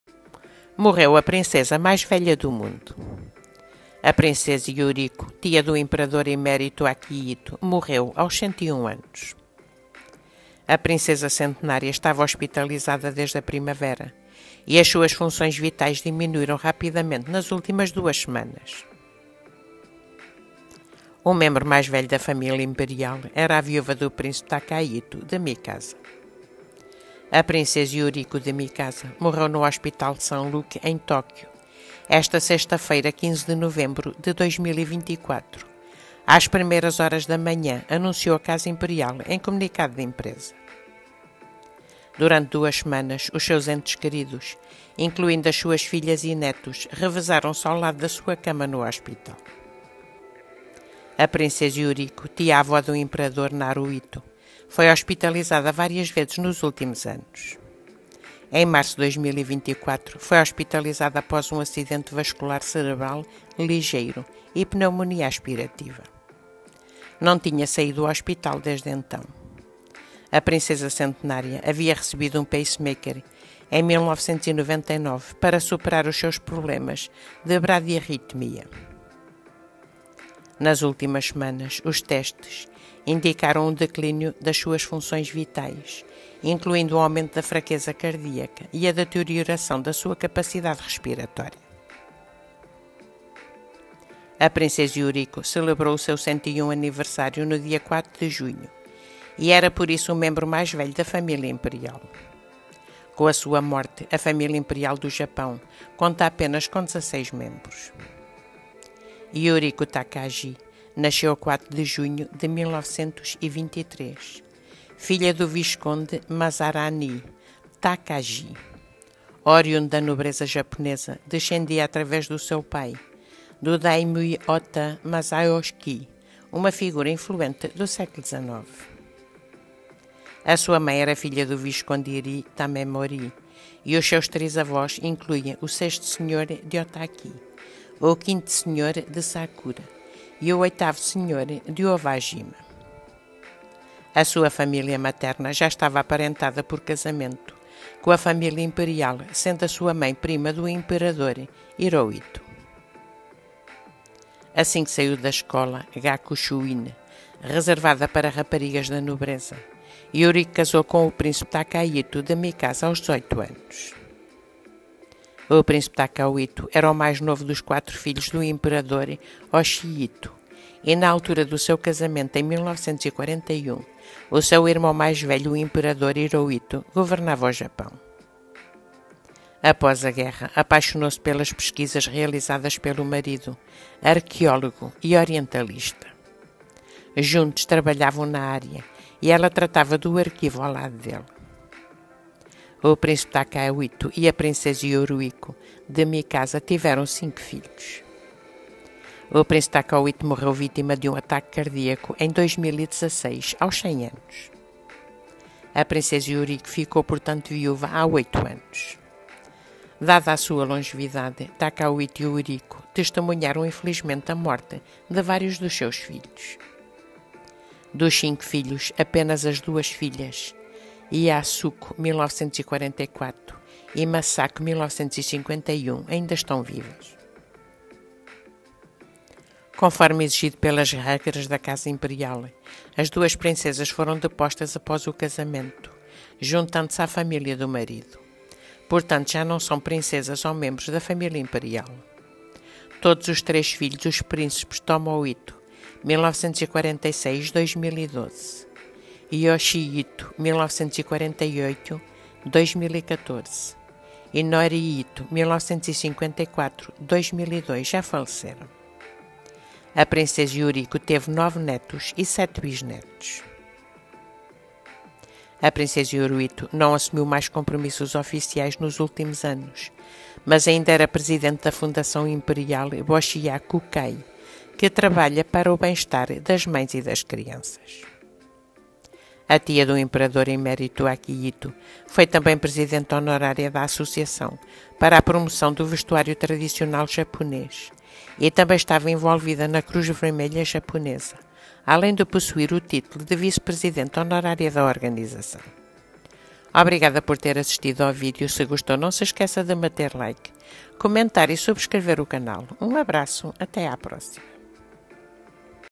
Morreu a princesa mais velha do mundo.
A princesa Yuriko, tia do imperador emérito Akihito, morreu aos 101 anos.
A princesa centenária estava hospitalizada desde a primavera e as suas funções vitais diminuíram rapidamente nas últimas duas semanas.